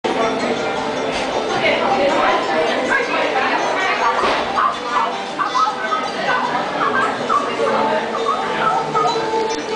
Georgia Aquarium - Mr Creepy Fish
This fish got the short end of the stick in the looks department. It's am arapaima. Translation = fugly fish